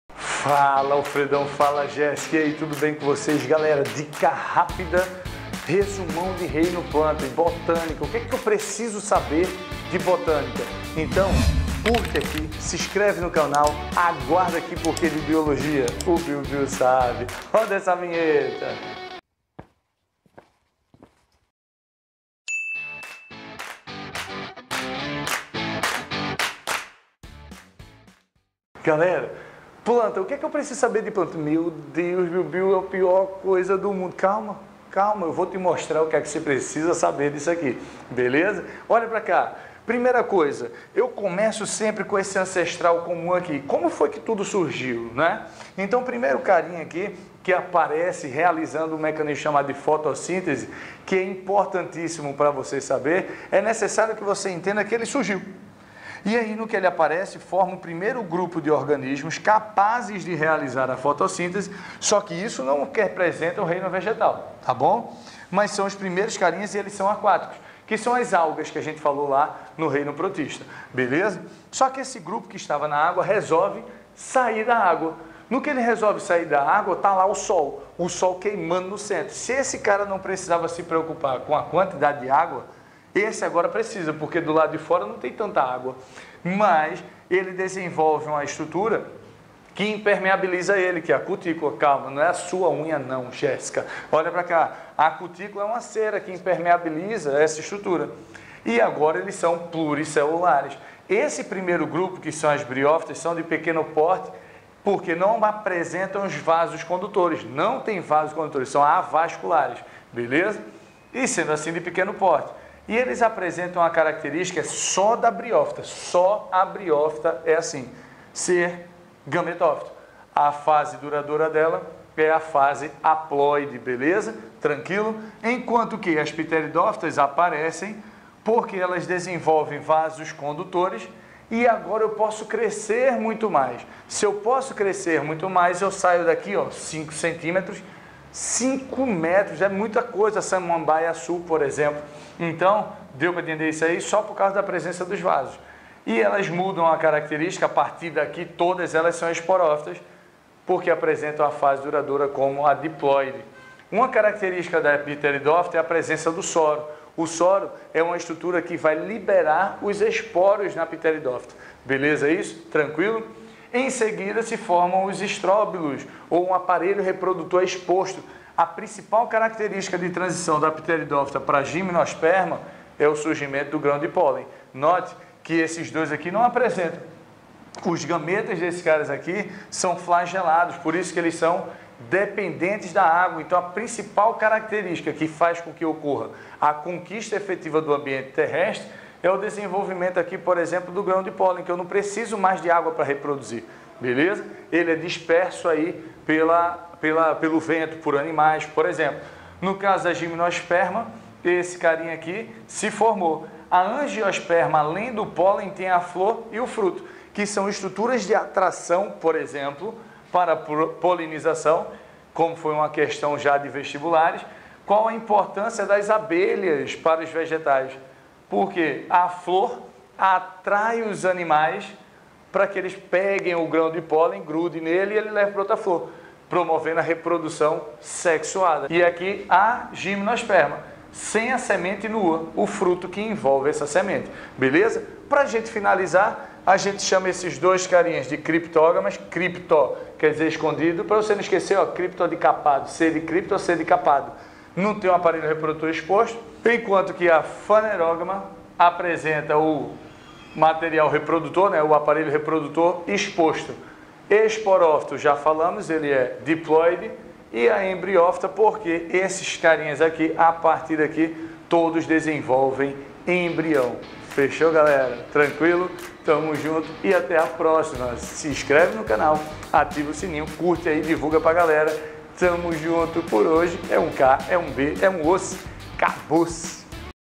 01 - Botânica - Caracteristicas gerais
Fala Alafredão, fala Jéssica, tudo bem com vocês?

Nessa aula maravilhosa, trabalharemos o reino plantae, abordando as características gerias de cada grupo. sempre de forma objetiva e rápida para otimizar seu tempo.

Então agora segura na cadeira que a lapada vai ser grande!!